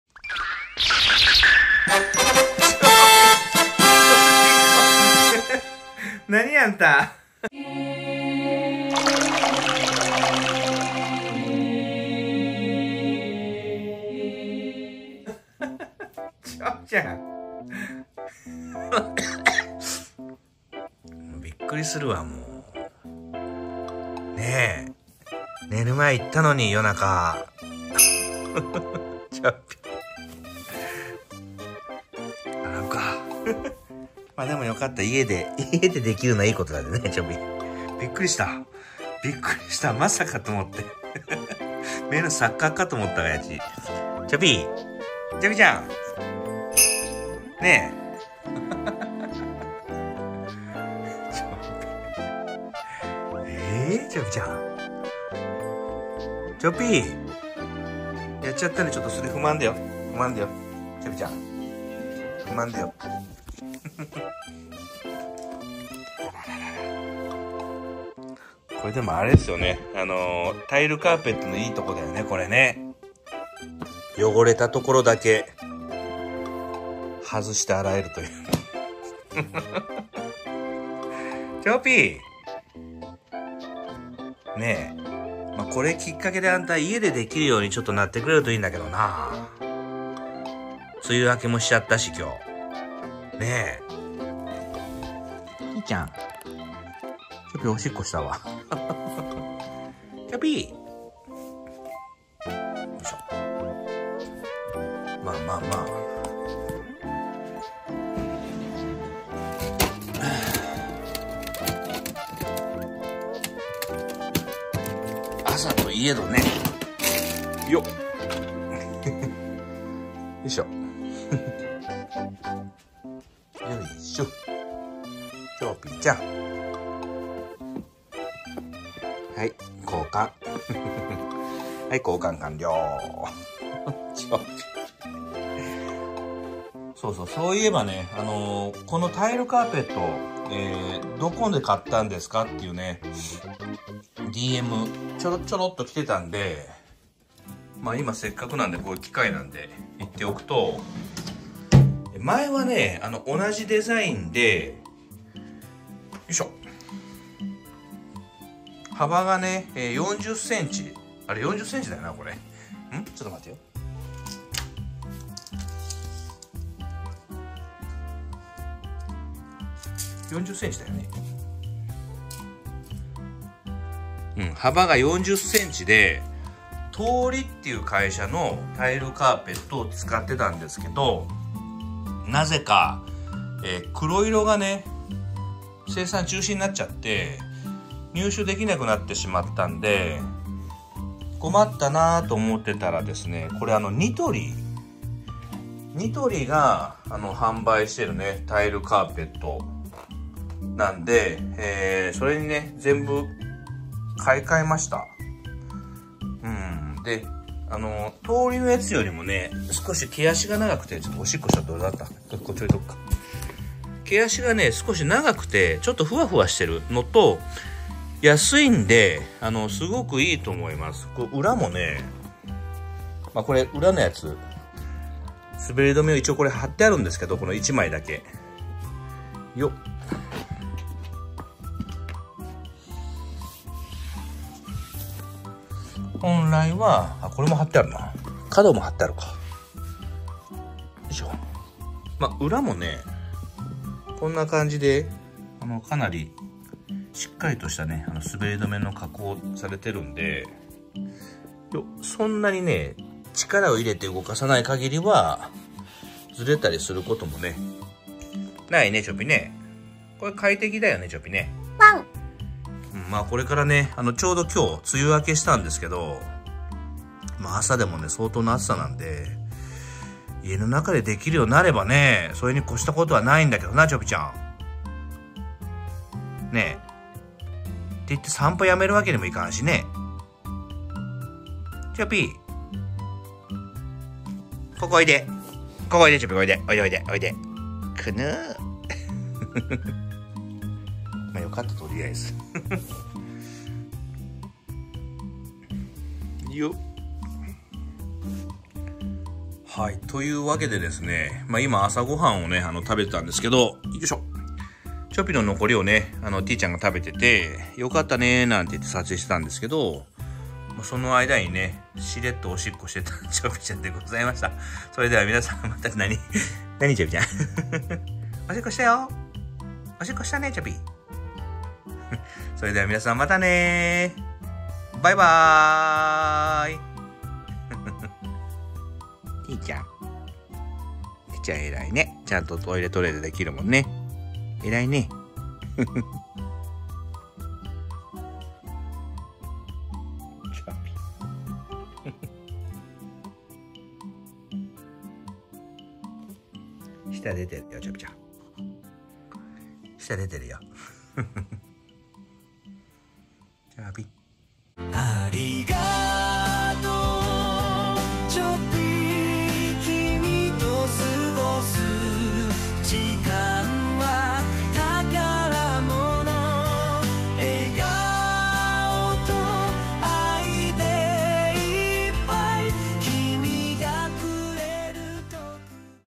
全く想像出来なかった高齢犬の行動に全米が泣くほどの衝撃が走りましたｗ
いやぁ、ホントにビックリしましたｗ
チョッピが御飯を食べ終え、残りを食べるティ～ちゃんをスマホで撮影していたんですが、ふとチョッピに目をやったら、既にシッコ体勢にｗｗｗ
加齢と共に近くなってきたので今は3～4回散歩に行っています。
前日は夜中の12時にも外へ出て済ませていたので、まさか！という感じでした(;'∀')

BGM
あみたろの声素材工房　小春音アミ